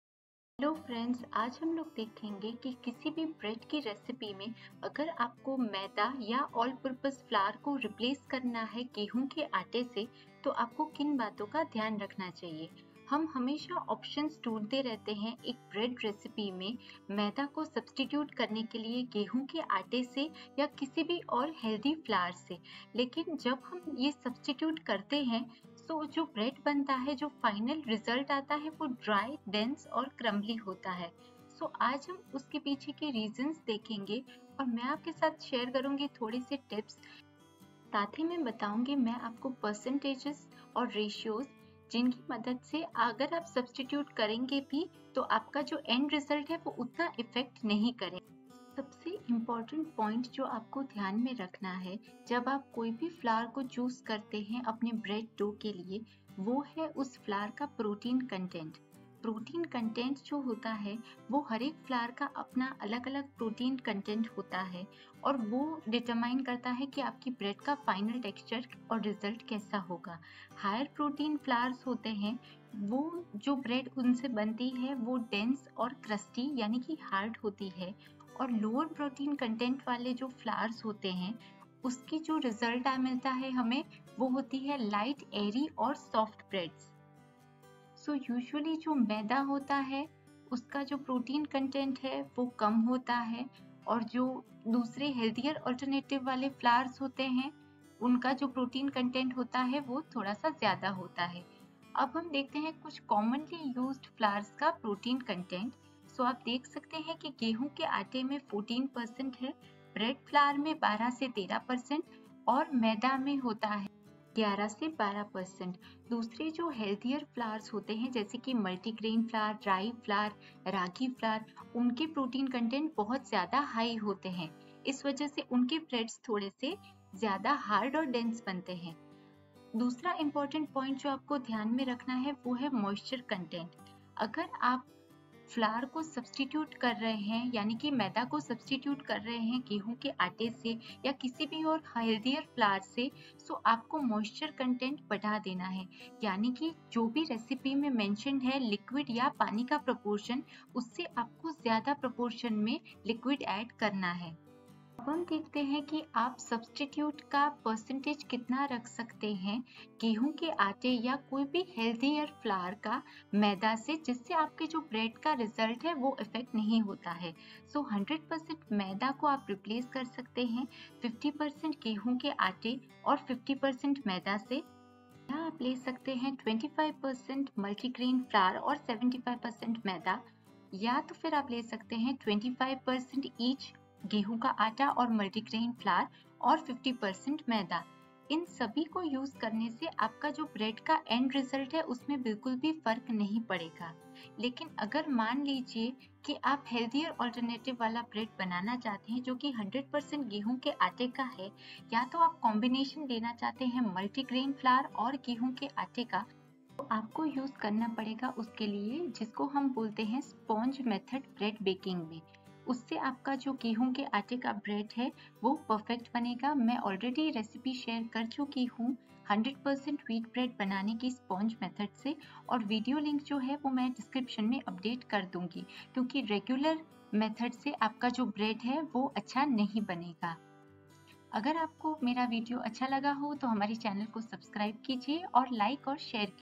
How to substitute healthy flours for all purpose flour in bread baking | in hindi
How to substitute healthy flours for all purpose flour in bread baking

We are always looking for options to substitute all purpose flour (or maida) in bread baking with whole wheat flour or any other healthier alternative. But, often we end up in getting a bread that is dry, dense and crumbly.

In this video, we will be sharing with you few tips which will help you to decide on flour substitutes effectively without compromising much on the end result.